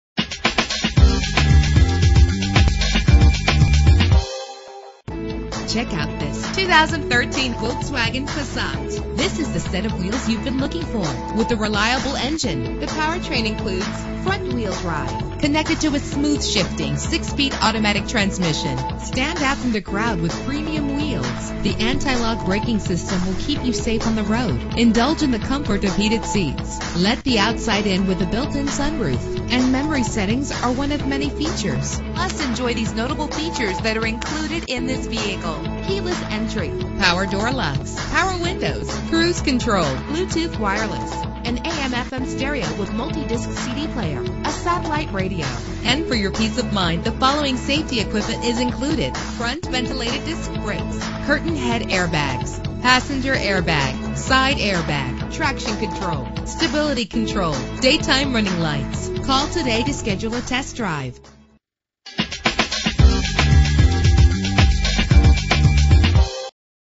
2013 Volkswagen Passat - Tallahassee FL
Year: 2013
Make: Volkswagen
Model: Passat
Trim: 2.5 SE
Engine: 2.5 liter inline 5 cylinder 20 valve
Transmission: 6-Speed Automatic
Color: Night Blue Metallic
Mileage: 0
Address:
3963 W. Tennessee Street